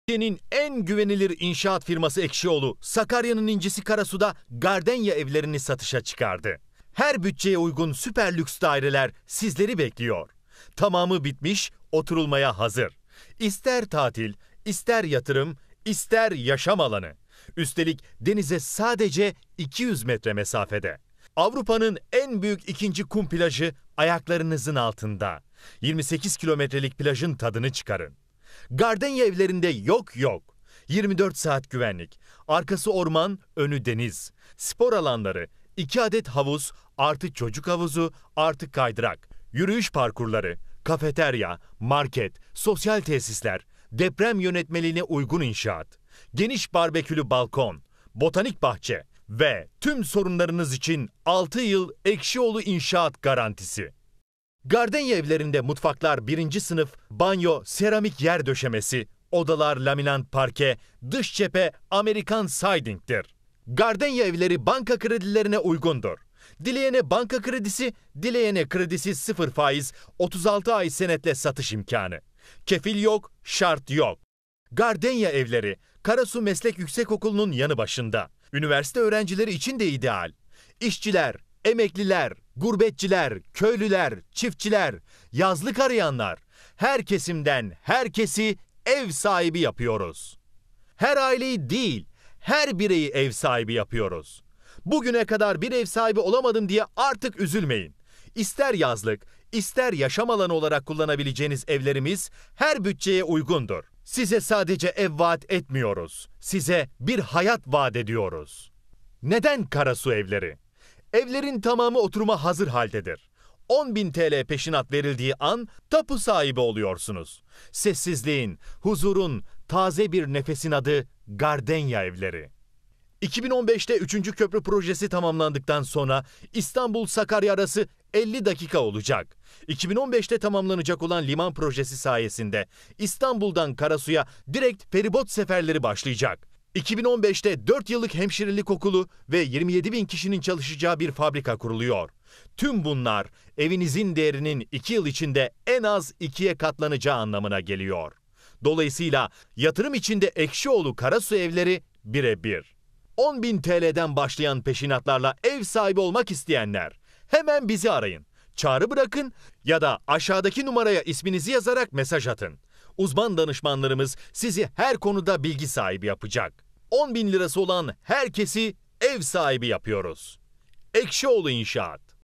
Ekşioğlu Evleri Karasu Gardenia
Ekşioğlu İnşaat Karasuda satılık daire ve yazlıklar. 10 bin Peşin Hemen taşın.Üstelik Sıfır Faiz imkanı ile. İster senet ister kredi. Ekşioğlu İnşaat Karasu'da herkezi daire sahibi yapıyor.Avrupa'nın en büyük ikinci kum plajına sahip olan Karasu, hem yatırım hem tatil diyor.